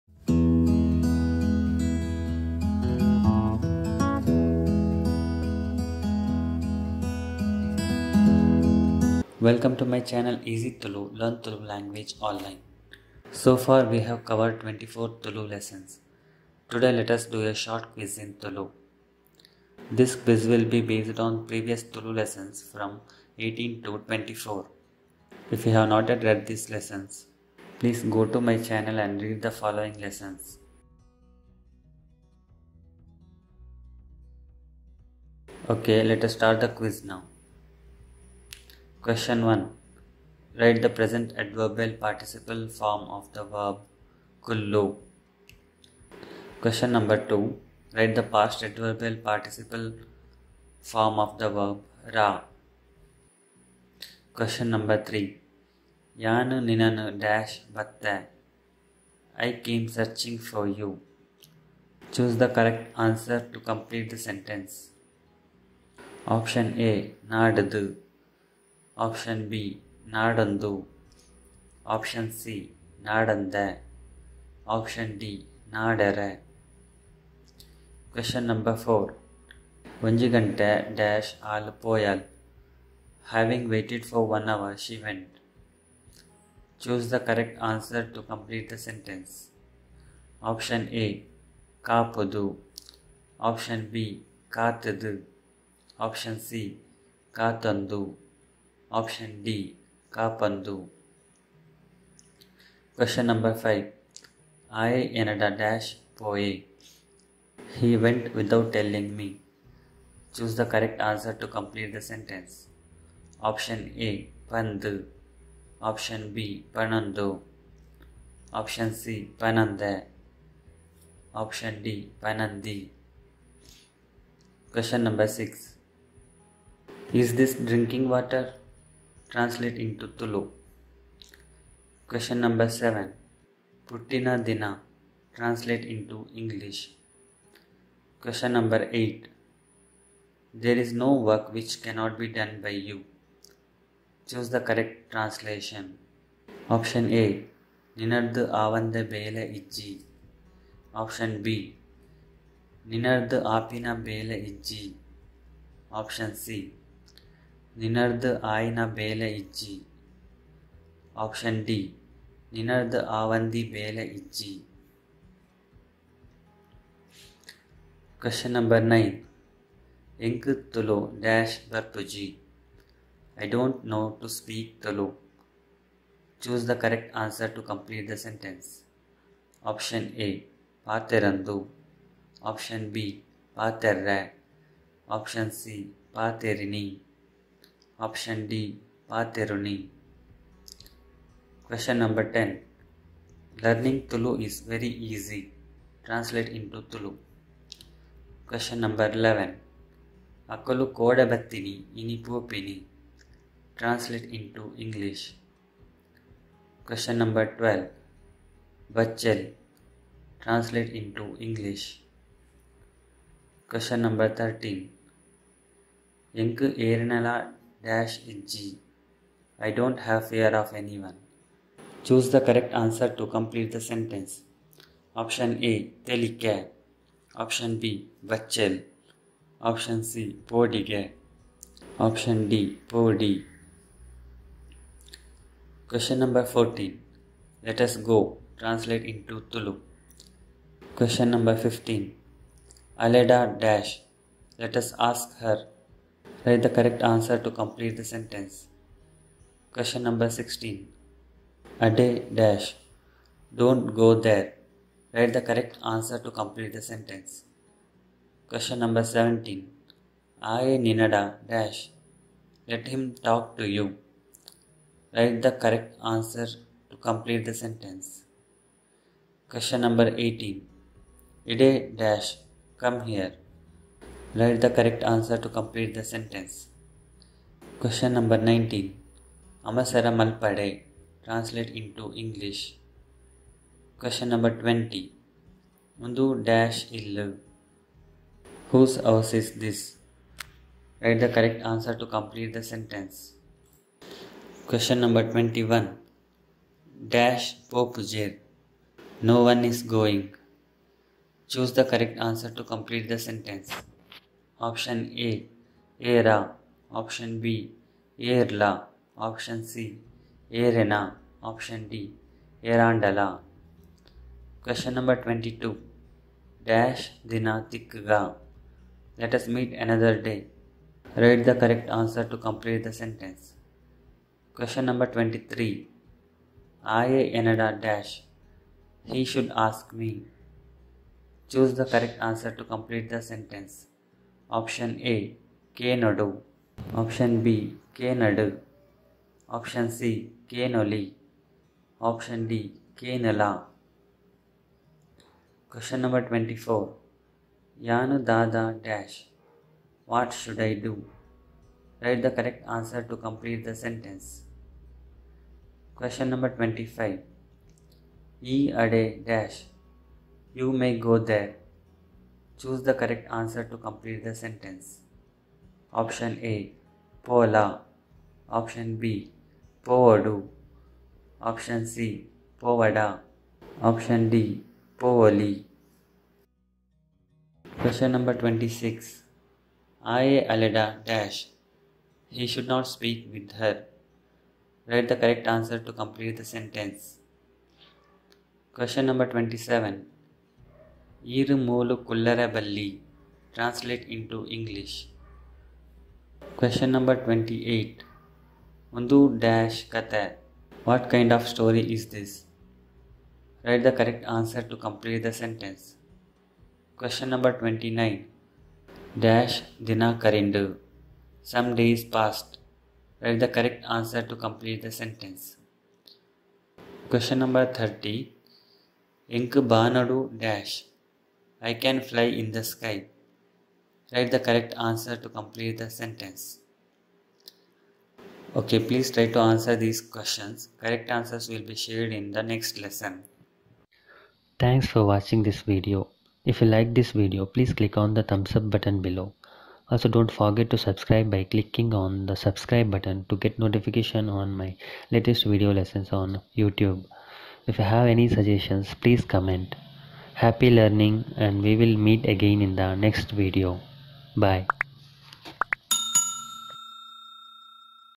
Tulu Practice - Quiz 4 | Learn Tulu through English
In this video you can find Tulu quiz.  This will help you to improve your knowledge on Tulu language. This quiz is based on previous Tulu lessons from 18 to 24. Here are the links.

The correct answers will be shared in the next video.
Happy Learning!